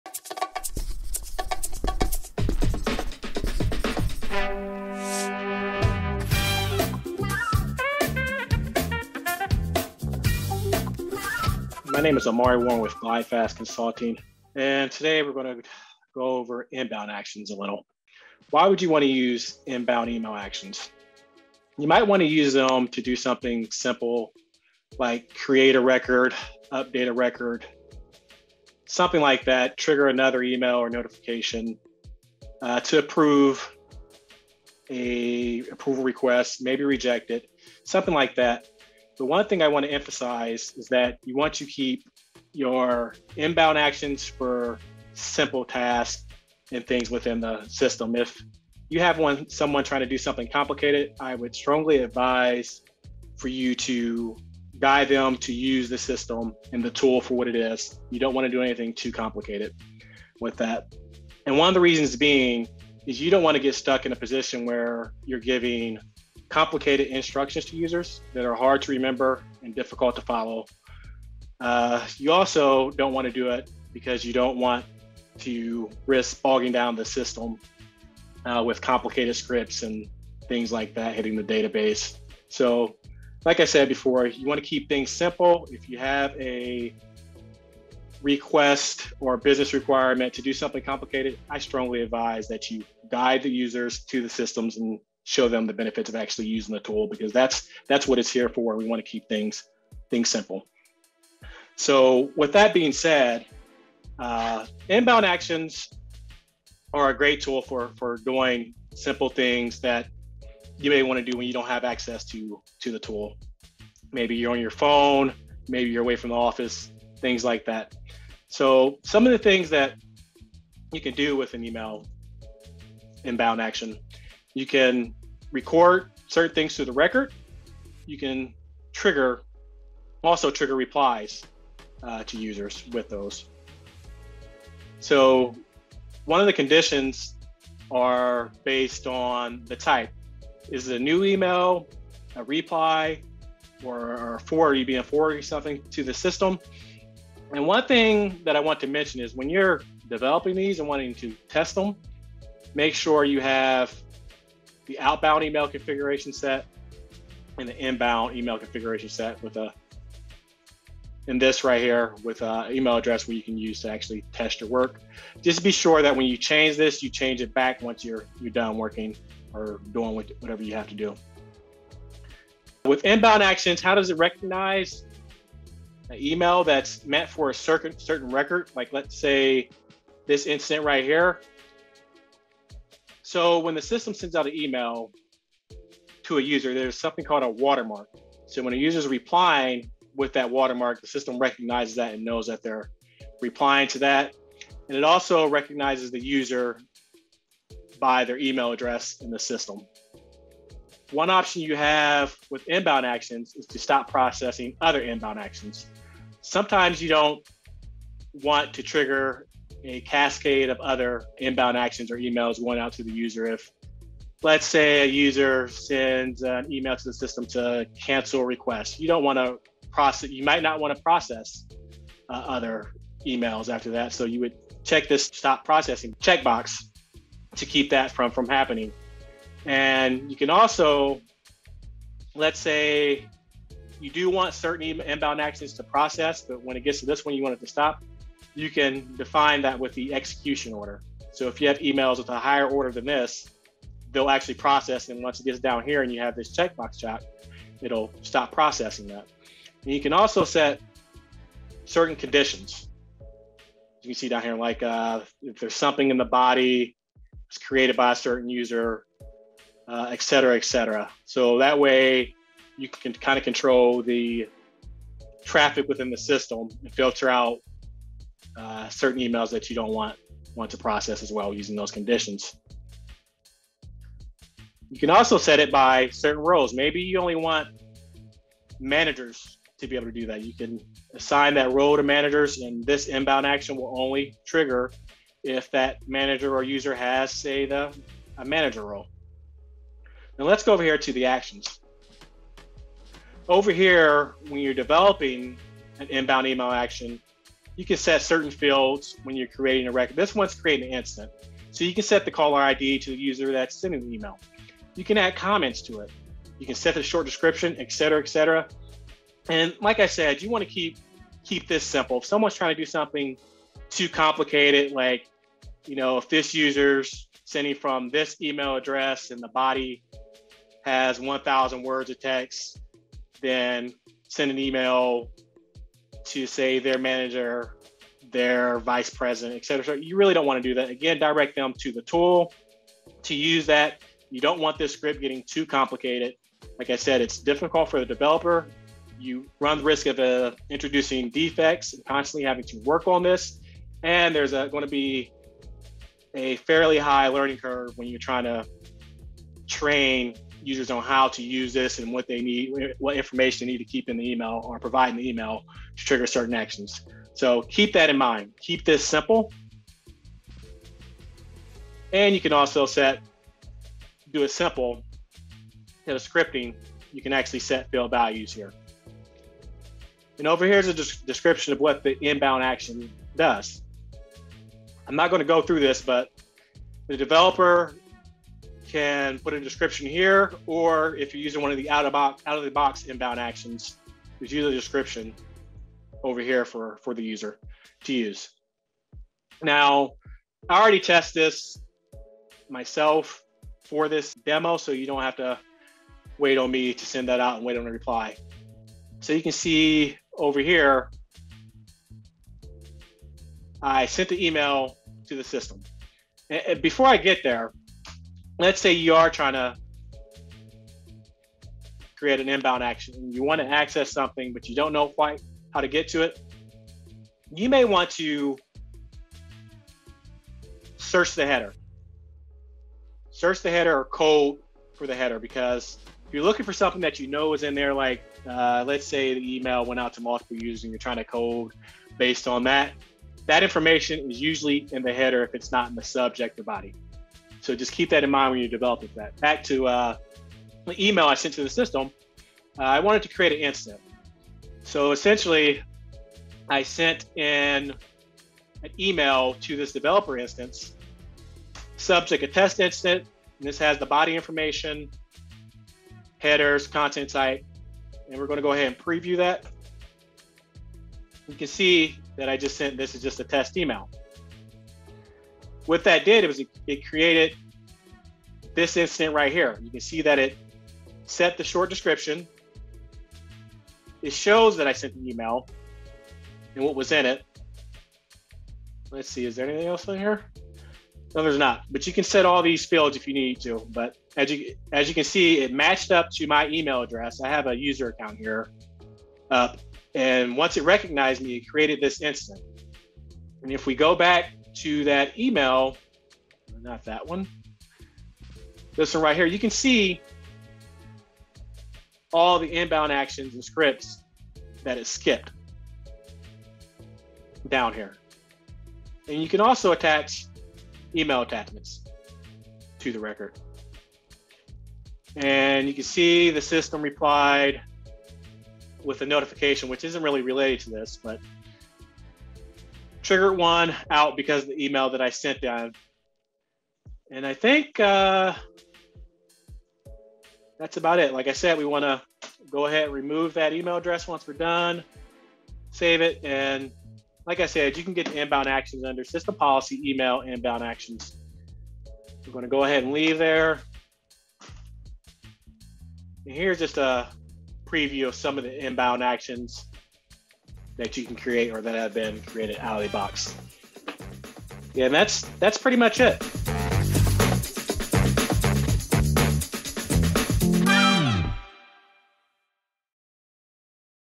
Inbound Email Actions | ServiceNow Tutorials
In this ServiceNow Tutorial, Omari Warren gives demonstration on Inbound Email Actions in ServiceNow.

Inbound email actions are similar to business rules: both use conditions and scripts that take action on a target table. An inbound email action checks the email for a watermark that associates it with a task and checks for other conditions.